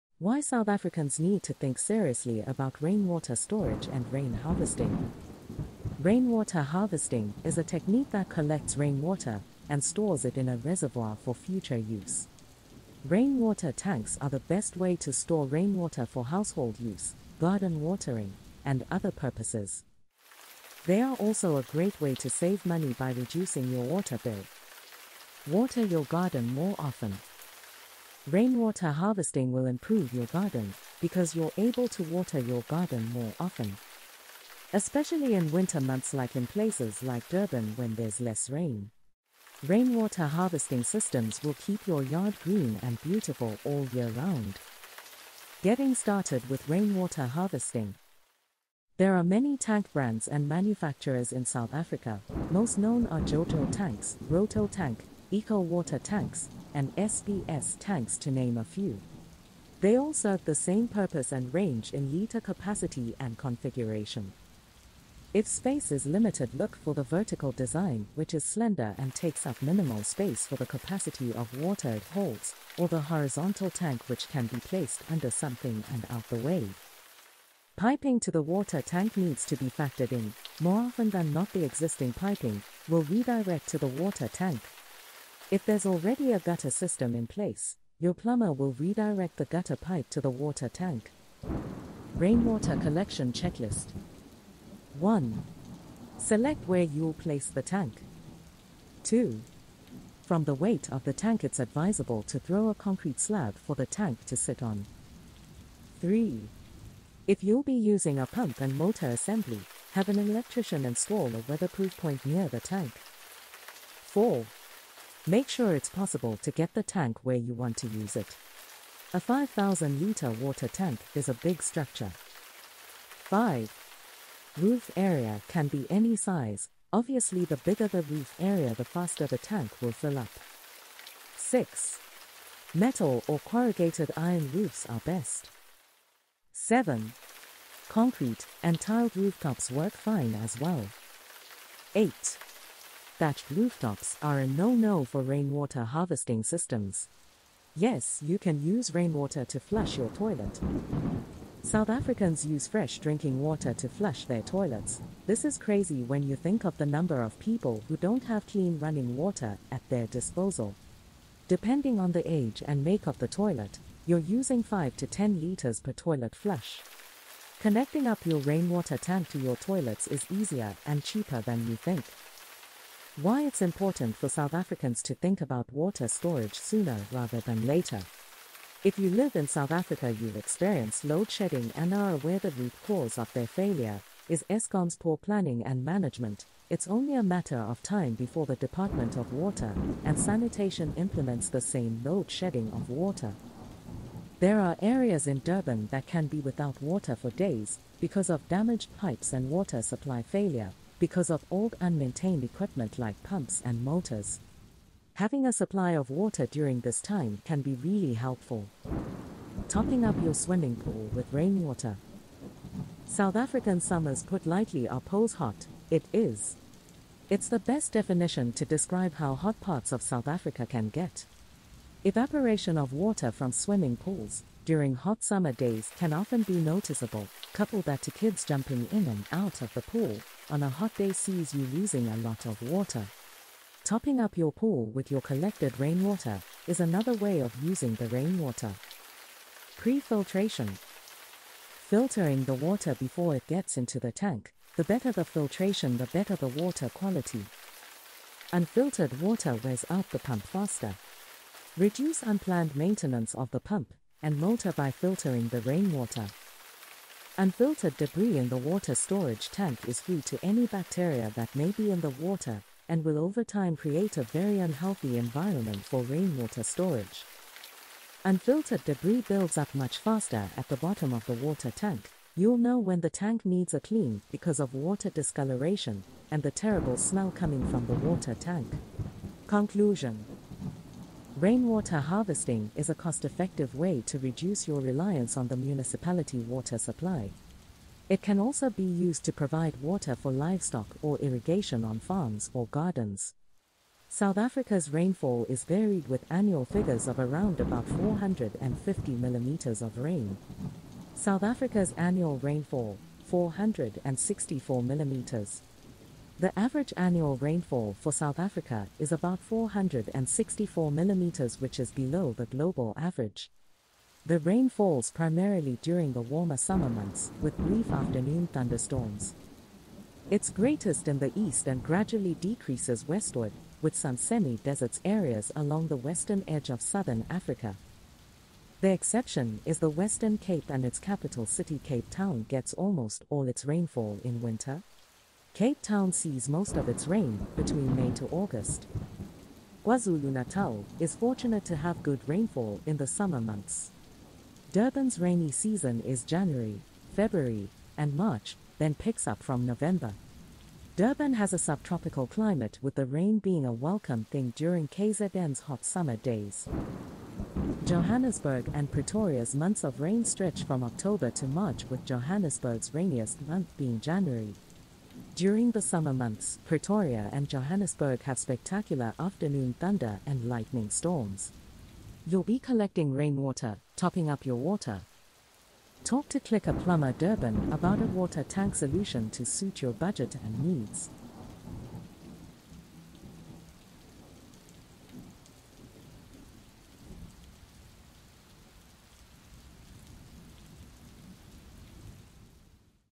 Rainwater Harvesting Explained 🌧
Why South Africans need to think seriously about rainwater storage and Rain Harvesting

Rainwater Harvesting is a technique that collects rainwater and stores it in a reservoir for future use. Rain Water Tanks are the best way to store rainwater for household use, garden watering, and other purposes.

They are also a great way to save money by reducing your water bill.

Water Your Garden More Often
Rainwater harvesting will improve your garden because you're able to water your garden more often. Especially in winter months like in places like Durban when there's less rain. Rainwater harvesting systems will keep your yard green and beautiful all year round.

Getting Started with rainwater harvesting
There are many tank brands and manufacturers in South Africa, most known are Jojo Tanks, Roto Tank, Eco Water Tanks, and SBS Tanks to name a few. They all serve the same purpose and range in liter capacity and configuration.

If space is limited look for the vertical design which is slender and takes up minimal space for the capacity of water it holds or the horizontal tank which can be placed under something and out the way

Piping to the water tank needs to be factored in, more often than not the existing piping will redirect to the Water Tank. If there's already a gutter system in place your plumber will redirect the gutter pipe to the water tank.

Rainwater Collection Check-List
1. Select where you'll place the tank

2. From the weight of the tank its advisable to throw a concrete slab for the tank to sit on

3. If you'll be using a pump and motor assembly have an electrician install a  weatherproof point near the tank

4. Make sure it's possible to get the tank where you want to use it. a 5000 Liter water tank is a big structure

5. Roof area can be any size obviously the bigger the roof area the faster the tank will fill up

6. Metal or Corrugated iron roofs are best

7. Concrete and Tiled rooftops work fine as well

8. Thatched rooftops are a No-No for Rainwater Harvesting systems

Yes, you can use rainwater to flush your toilet.
South Africans use fresh drinking water to flush their toilets, this is crazy when you think of the number of people who don't have clean running water at their disposal. Depending on the age and make of the toilet, you're using 5 to 10 liters per toilet flush. Connecting up your rainwater tank to your toilets is easier and cheaper than you think

Why it's important for South Africans to think about water storage sooner rather than later.
If you live in South Africa you've experienced load shedding and are aware the route cause of their failure is Eskom's poor planning and management, it's only a matter of time before The Department of Water and Sanitation implements the same load shedding of water.

There are areas in Durban that can be without water for days because of damaged pipes and water supply failure because of old unmaintained equipment like pumps and motors. Having a supply of water during this time can be really helpful
Topping Up Your Swiming Pool with Rainwater
South African summers put lightly are poes hot, it is. It's the best definition to describe how hot parts of South Africa can get. Evaporation of water from swimming pools during hot summer days can often be noticeable, couple that to kids jumping in and out of the pool on a hot day sees you losing a lot of water. Topping up your pool with your collected rainwater is another way of using the rainwater

South Africa’s Annual Rainfall: 464mm
The average annual rainfall for South Africa is about 464 mm which is below the global average. The rain falls primarily during the warmer summer months, with brief afternoon thunderstorms.

It's greatest in the east and gradually decreases westward, with some semi-deserts areas along the western edge of Southern Africa.

The exception is the Western Cape and its capital city Cape Town gets almost all its rainfall in winter. Cape Town sees most of its rain between May to August

KwaZulu Natal is fortunate to have good rainfall in the summer months. Durban's rainy season is January, February, and March, then picks up from November. Durban has a subtropical climate with the rain being a welcome thing during KZN's hot summer days

Johannesburg and Pretoria's months of rain stretch from October to March with Johannesburg's rainiest month being January. During the summer months, Pretoria and Johannesburg have spectacular afternoon thunder and lightning storms. You'll be collecting rainwater, topping up your water
Talk to Click a Plumber Durban about a water tank solution to suit your budget and needs